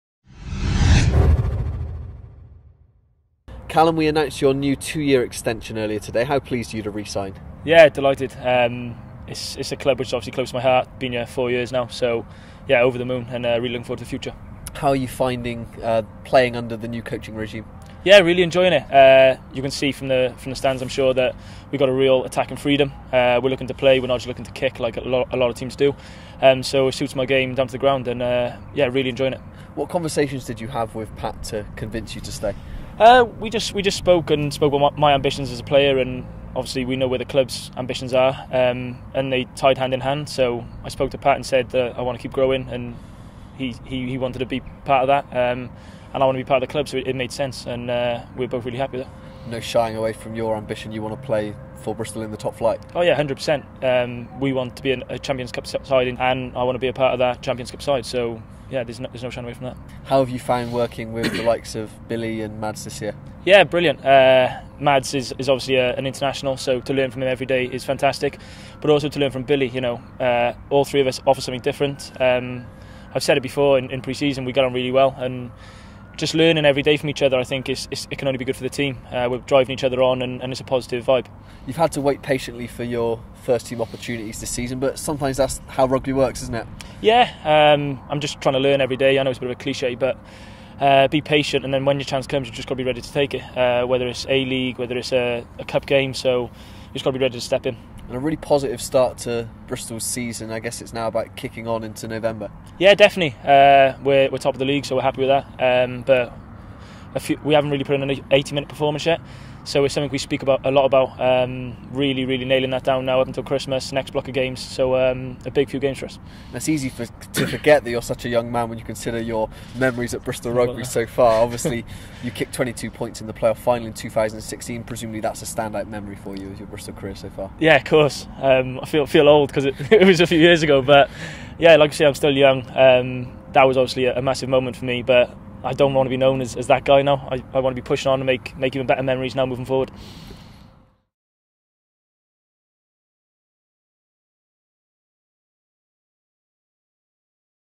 Sheedy delighted to pen new contract with Bristol Rugby
Callum Sheedy talks to Bristol Rugby TV after signing a new, two year deal with the club.